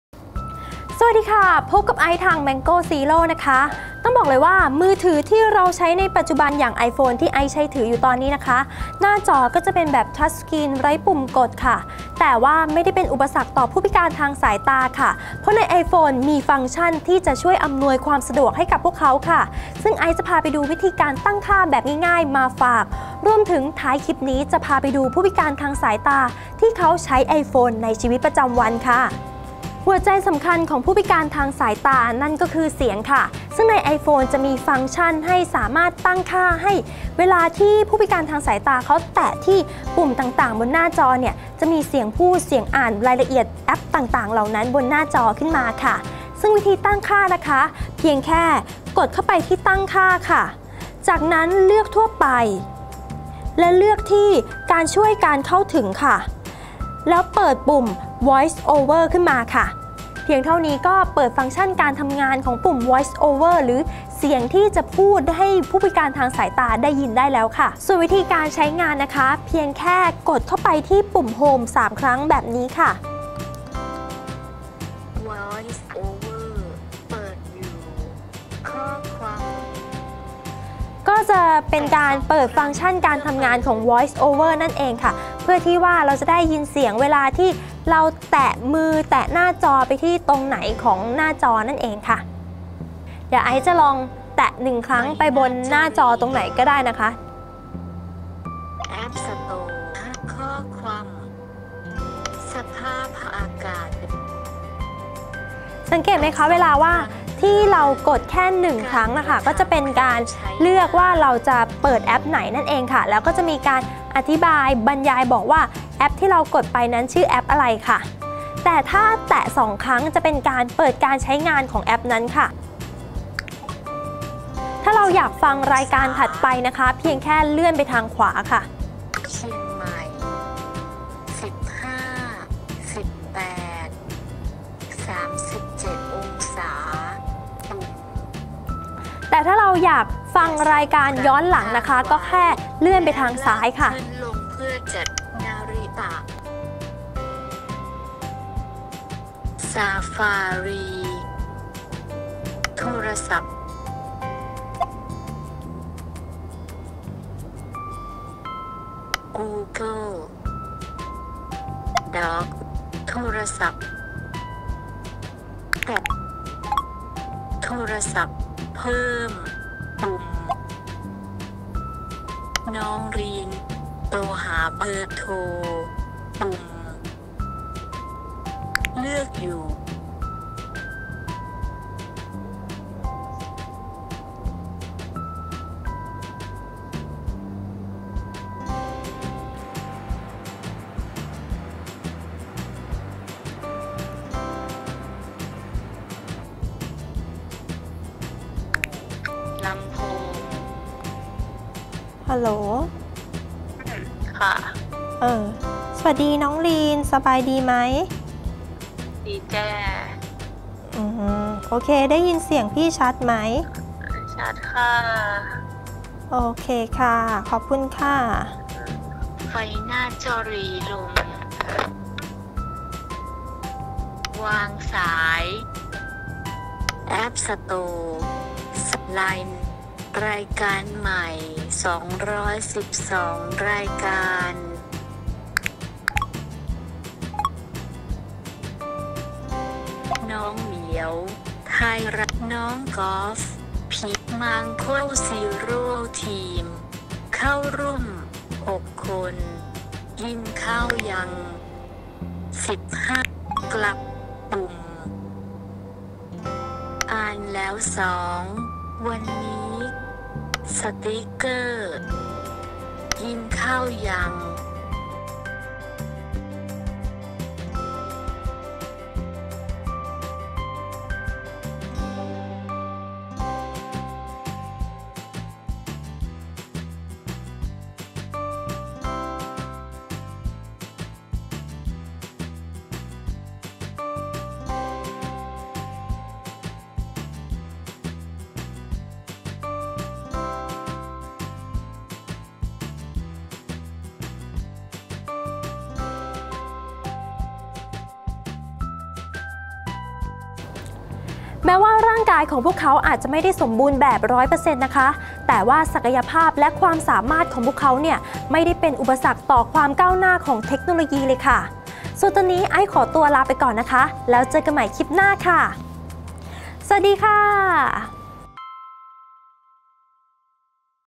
วิธีตั้งค่า iPhone สำหรับผู้พิการทางสายตา
มือถือที่ใช้กันอยู่ในปัจจุบัน ส่วนใหญ่จะเป็นหน้าจอทัชสกรีนไร้ปุ่มกด โดยเฉพาะไอโฟน เพื่อให้ใช้งานได้หลากหลาย   แต่ก็ไม่ได้เป็นอุปสรรคสำหรับผู้พิการทางสายตาค่ะ เพราะในไอโฟนมีฟังก์ชั่นที่มาช่วยอำนวยความสะดวกให้กับพวกเขา สามารถแชทไลน์ เล่นเฟซบุ๊กได้ตามปกติเลย ตามทีมงาน Mango Zero ไปดูวิธีตั้งค่าง่ายๆมาฝากกันค่ะ
นอกจากนี้เรายังได้ไปพบกับผู้พิการทางสายตาที่เขาใช้ไอโฟนเป็นประจำ สำหรับเข้าใช้งานต่างๆมาด้วยค่ะ